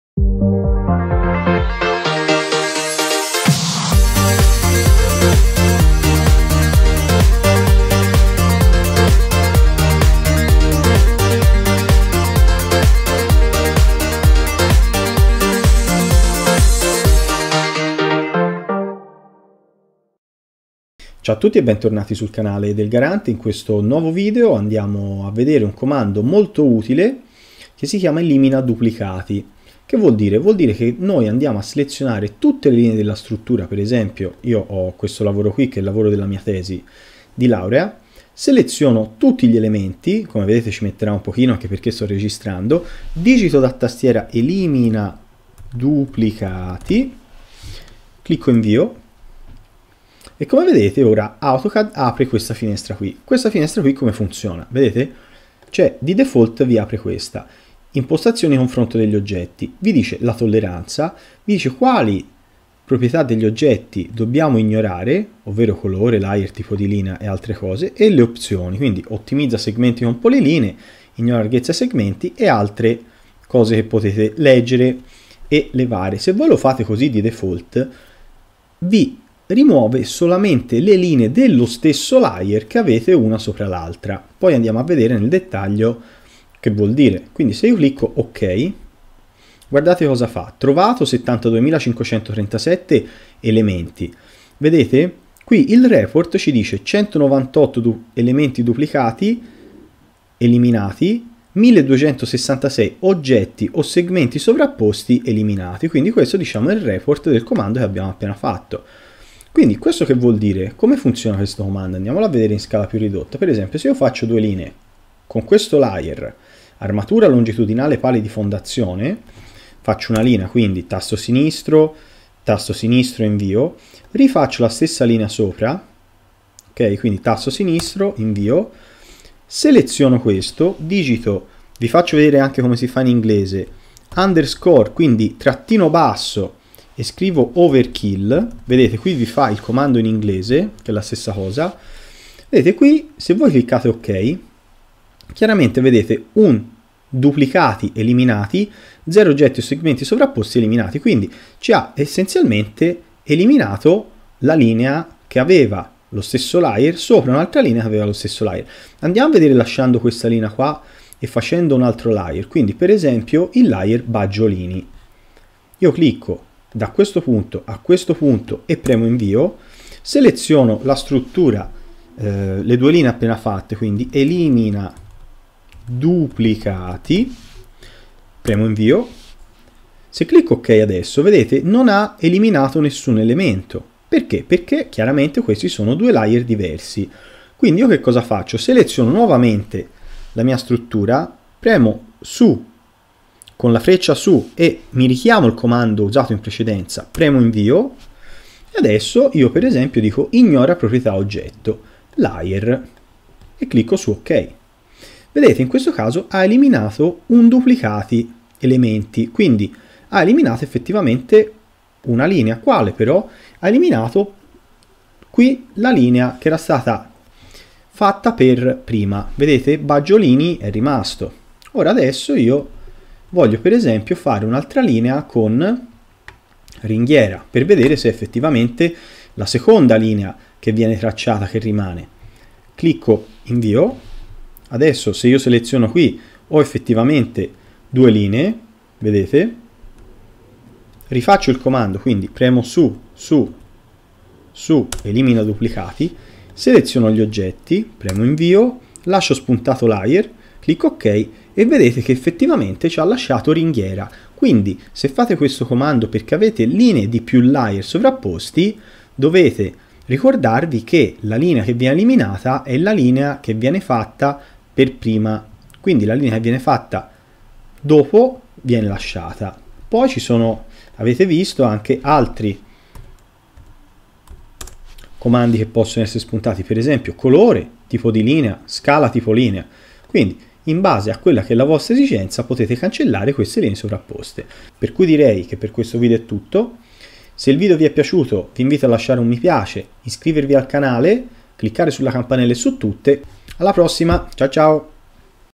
Tutorial AutoCAD - Cancellare linee doppie sovrapposte ELIMINADUPLICATI
In questo video vediamo come cancellare linee sovrapposte di stesso layer e layer diversi, attraverso il comando ELIMINADUPLICATI.